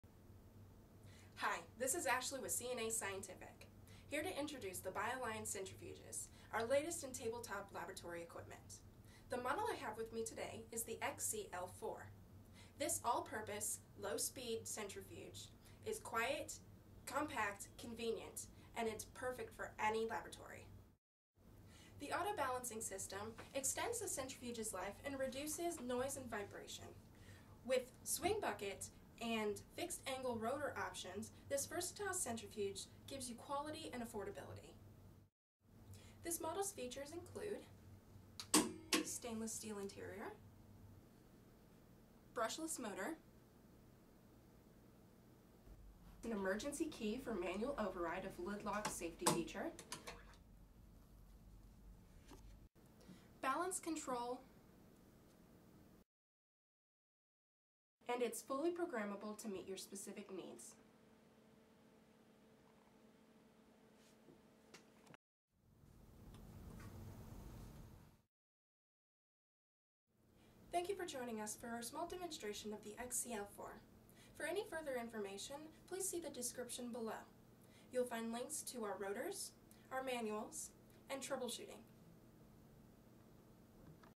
Bio Lion XC-L4 Overview
This all purpose low speed centrifuge is quiet, compact, and convenient which makes it ideal for any lab. The auto-balancing system extends the centrifuge life and reduces noise and vibration. With a swing bucket and fixed angle rotor option this versatile centrifuge gives you quality and affordability.

Troubleshooting by Error Message:

E1 - Imbalance:
• The weights of the tubes are not equally balanced.
• Rotor is not installed or spinning correctly
• Bent or damaged motor shaft.
Solutions: Make sure the tubes are equally balanced as described in the "Caution" section. If it is the Rotor, please double check that everything is assembled correctly as described in TABLE III. If there is any damage, please report to your dealer.

E2 - Over speed:
• Power supply isn't correct.
• Failing speed sensor
Solutions: Check to make sure the voltage and frequency of your outlet are correct, if not, unplug immediately. For any failing equipment, please report to your dealer.

E3 -- Lid:
• Lid not closed completely.
• The lid protection device is not working.
Solutions: Make sure there is nothing blocking, or jamming the lid; keeping it from closing. For any damaged parts, please report to your dealer.

E4 - Electronic plate failure:
• Failed electronic plate.
Solutions: Check out to insure the plug is in good working condition. A failed Electronic plate needs to be replaced. Please report to your dealer.

E5 - Motor failure:
• The connecting line between motor and electronic plate is loose.
• Motor has failed.
Solutions: These failures should be fixed by a professional. Please report to your dealer.

E8 - Setup failure:
Solutions: Reset the setting in assembly and operation part of manual.

Display is not working
• Unit is not turned on.
• Check for secure connection of the power cord at the unit back and at the power outlet.
• Blown fuse, please report to your dealer.